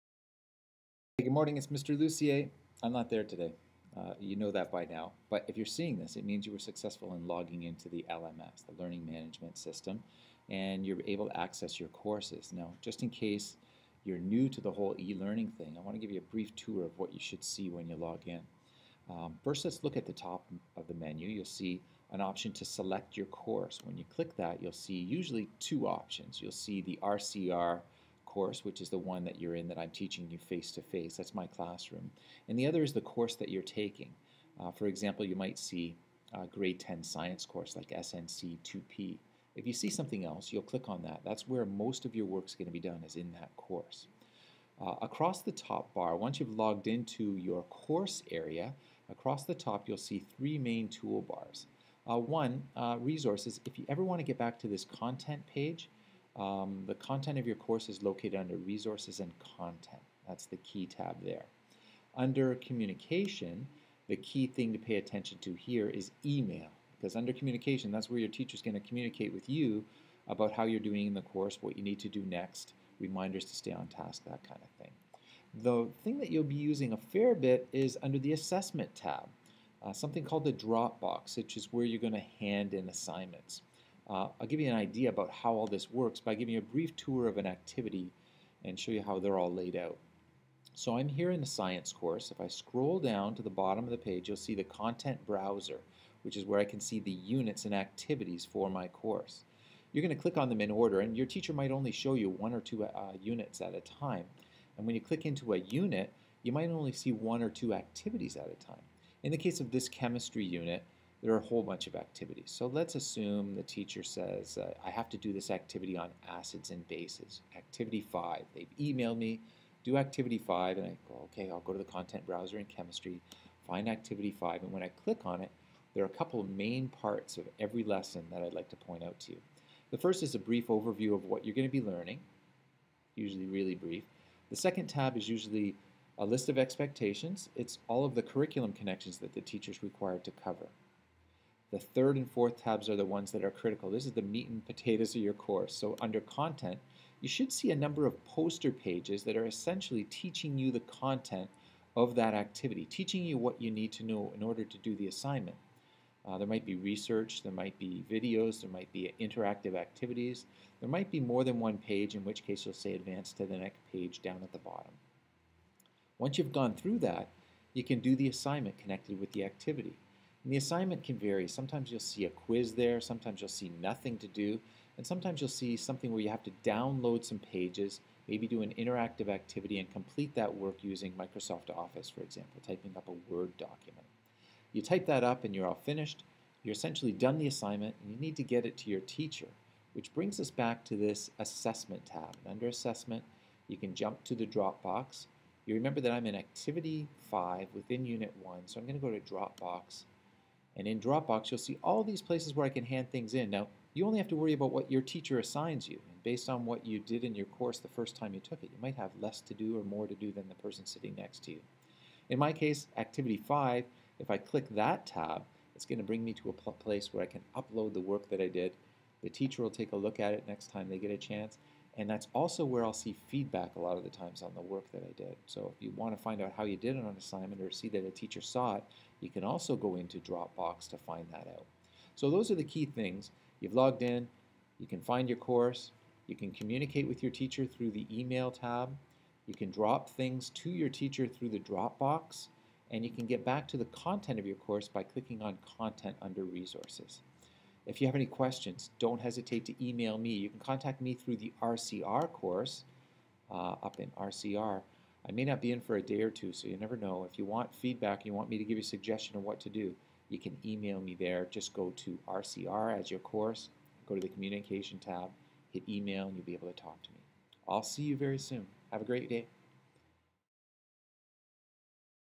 Intro to Credit Recovery Courses
a brief overview for students new to credit recovery in the LDCSB